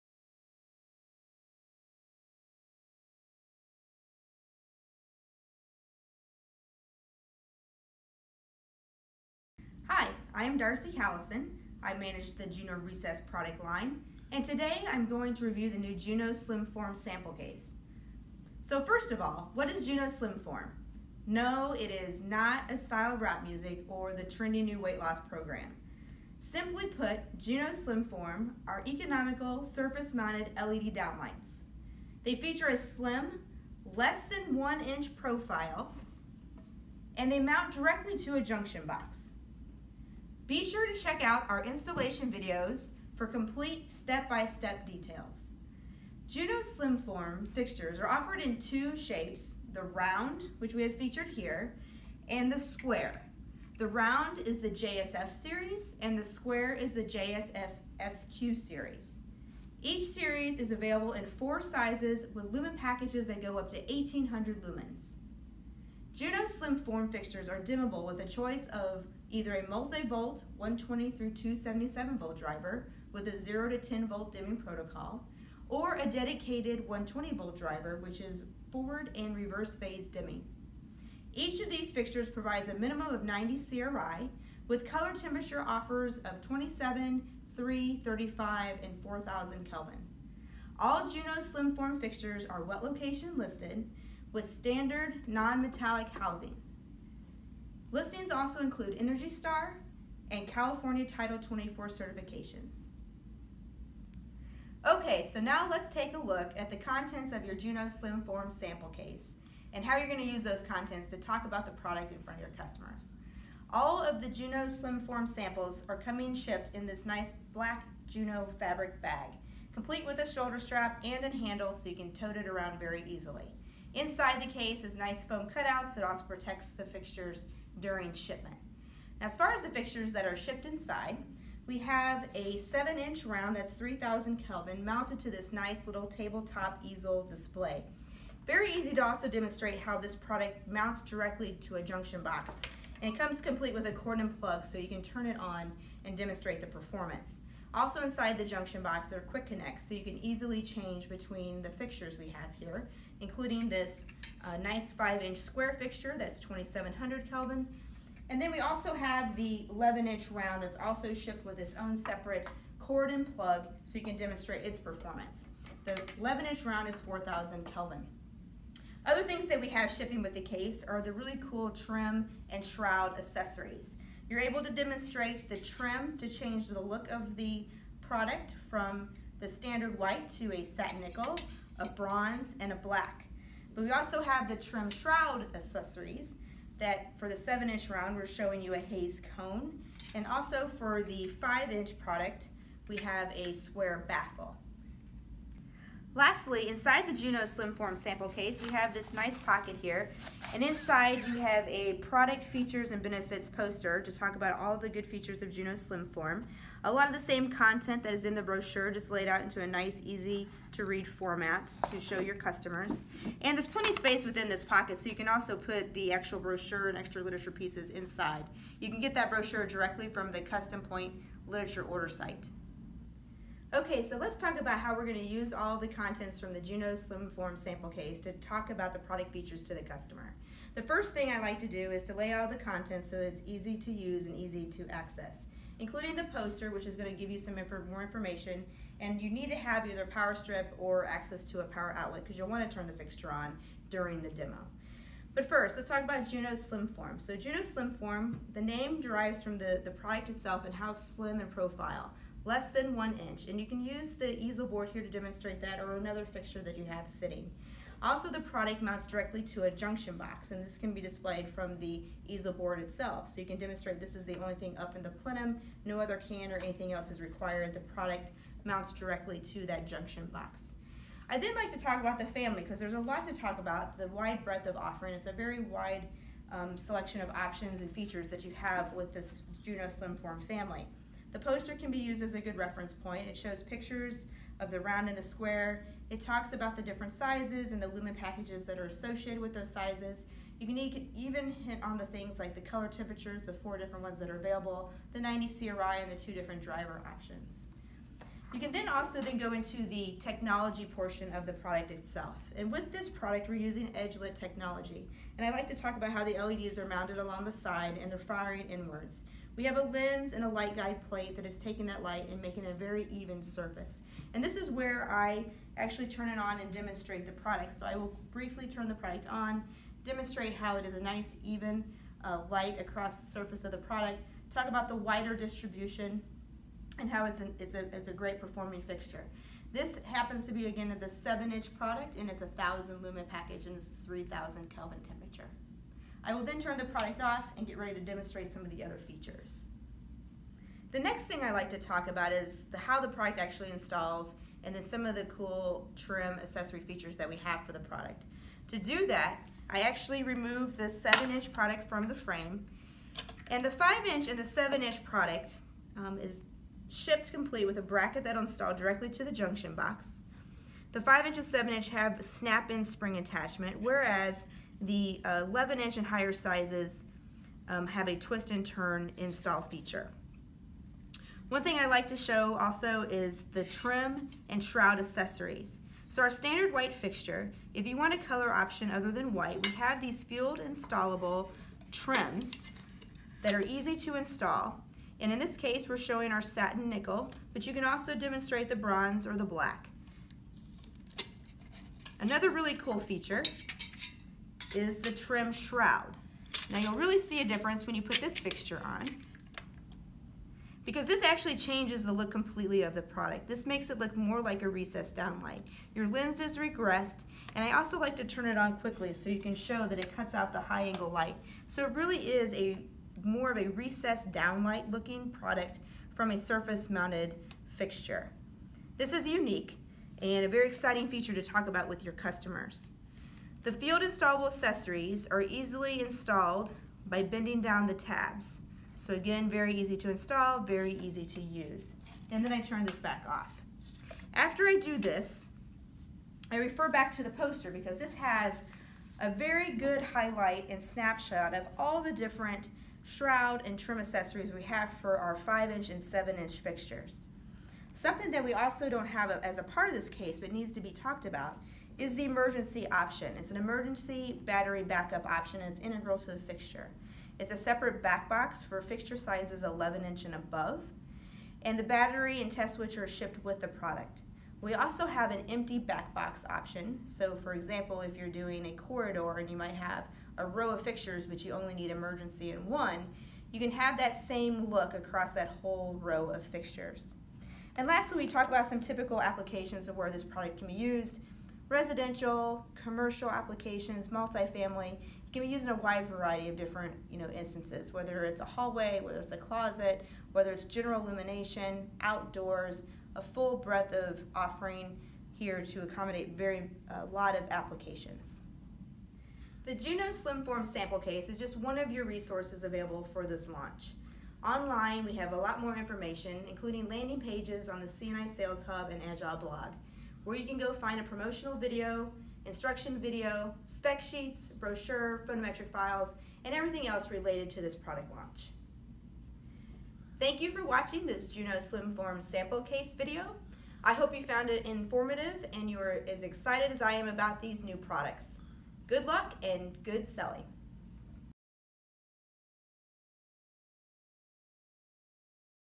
Juno SlimForm™ LED Sample Case Video
A comprehensive review of the Juno SlimForm™ sales sample case contents and key selling features from Product Manager, Darcie Callison.